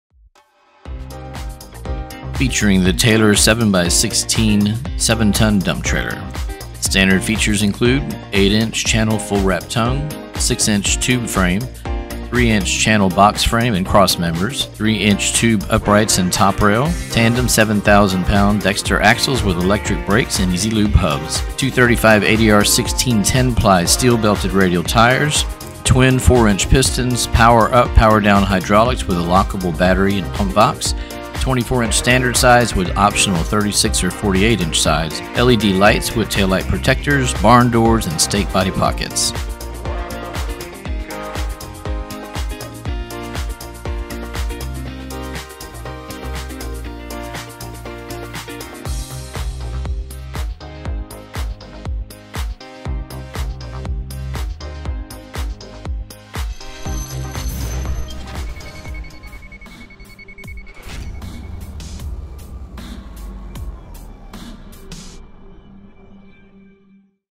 7x16 TAYLOR TRAILERS DUMP Trailer offered by Load Runner Trailers
Sales | Parts | Service
We've got you covered with over 1.000 Trailers in Stock!

TRAILER SPECIFICATION

MODEL: D82-16T7-24S
TRAILER TYPE: Dump
SIZE: 7x16
GVWR Trailer Only: 14000
GVWR Including Tongue Weight: 15400
# AXLES: 2
AXLE CAPACITY: 7000
AXLES WITH BRAKES: 2
AXLES TYPE: Spring
CONSTRUCTION: Steel
LENGTH: 16 ft
USABLE WIDTH: 82 in
PULL TYPE: Bumper
RAMP TYPE: